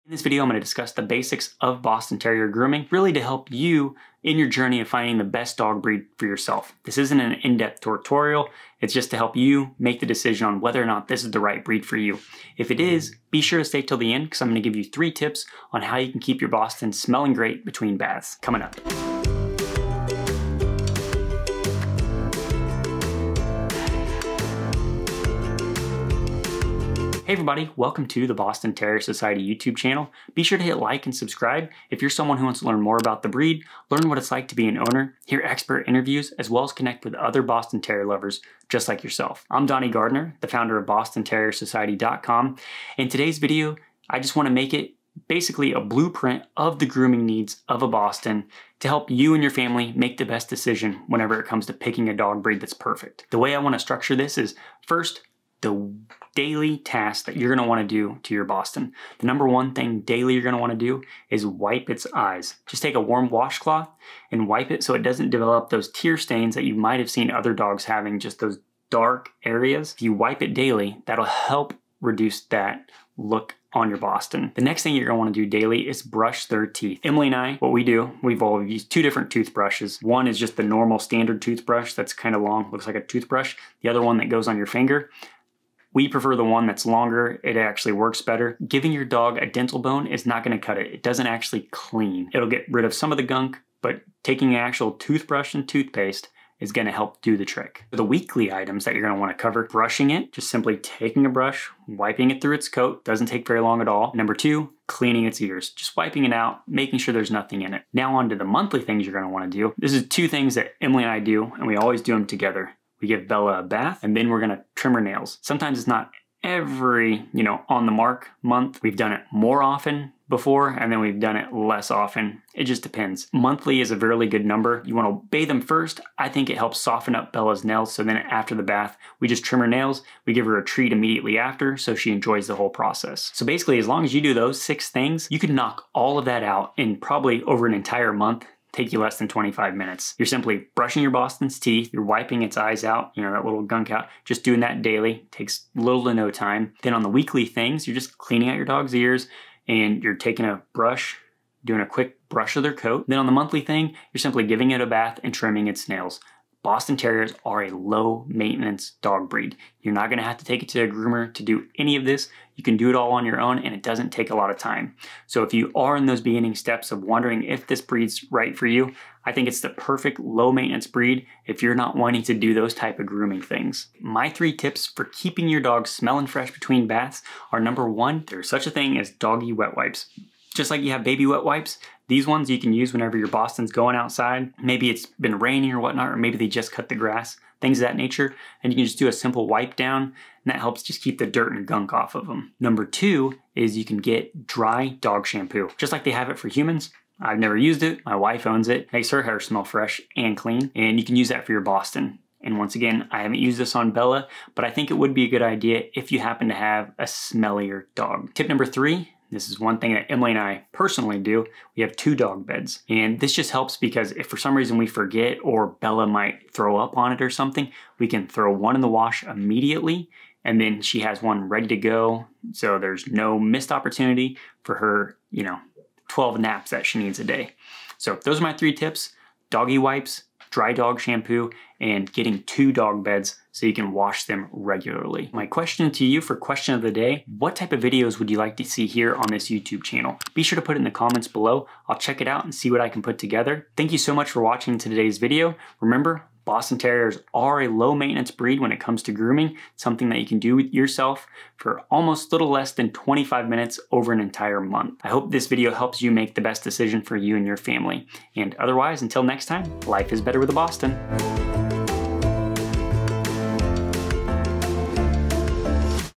Basic Grooming Needs Of A Boston Terrier
What are the basic grooming needs of a Boston Terrier?

This video is for someone who is thinking about getting a Boston Terrier for the first time. This is not an in-depth how-to groom a Boston Terrier but more of an overview of what you can expect the grooming requirements of a Boston Terrier puppy will be. In this video, I cover: how often to bathe a Boston Terrier; how often to brush a Boston Terrier; how often to clean your Boston in general.

Boston Terrier Society’s Favorites

Food

Grooming

Health

Comfort

Basics